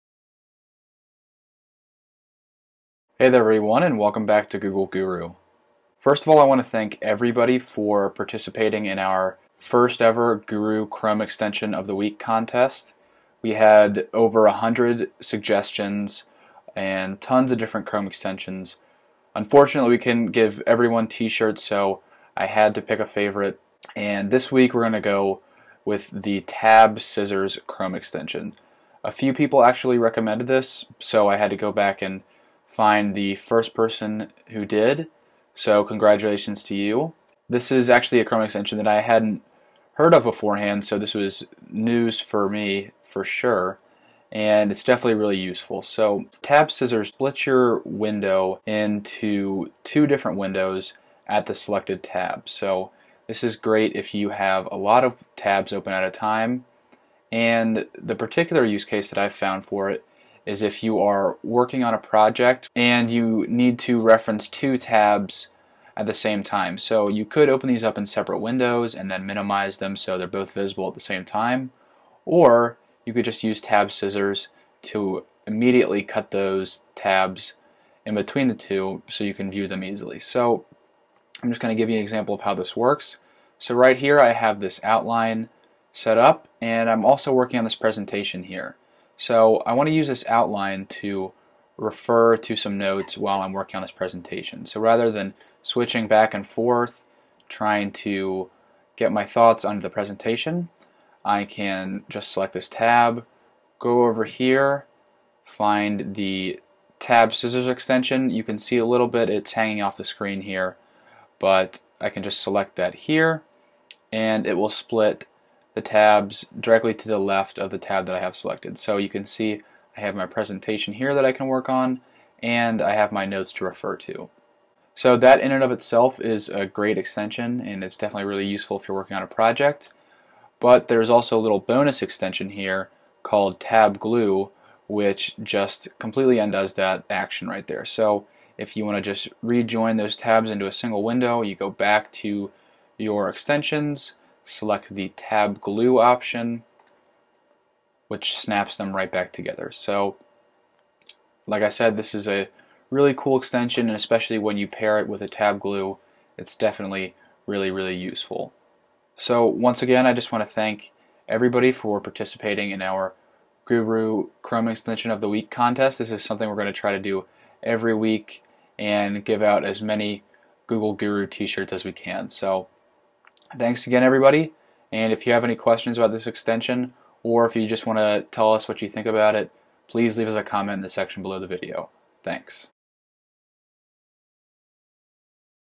Tab Scissors and Tab Glue
Our very first Gooru Chrome Extension of the Week winner actually suggested two extensions that work seamlessly together. Here's what our winner had to say about Tab Scissors and Tab Glue:

"Ever need to split your screen so you can work on 2 documents at once? Tab Scissors is amazing! Ready to have 1 screen back? Of course, Tab Glue does the trick!"

Tab Scissors and Tab Glue are both available in the Chrome Webstore and are two of our favorite productivity extensions. If you find yourself constantly switching back and forth between windows / tabs, this is definitely something worth checking out!